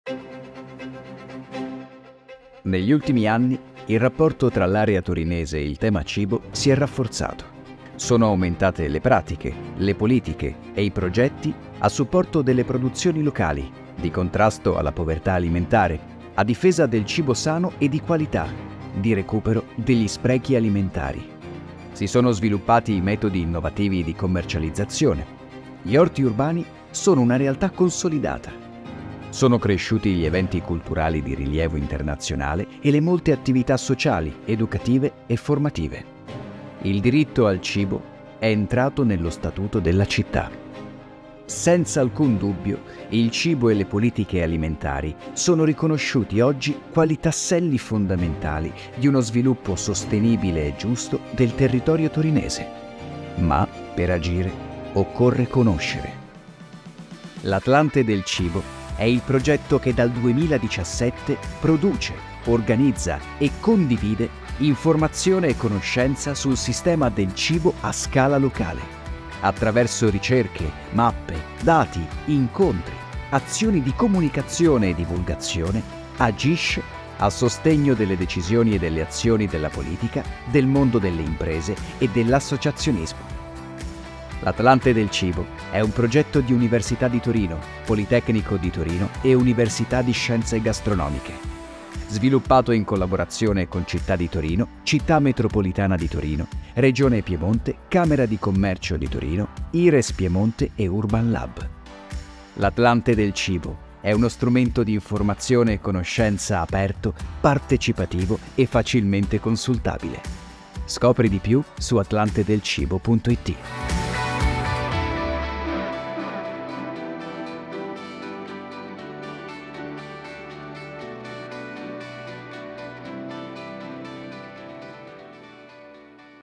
Atlante del Cibo di Torino Metropolitana
Costruire insieme azioni e conoscenza sul sistema del cibo di Torino e del territorio metropolitano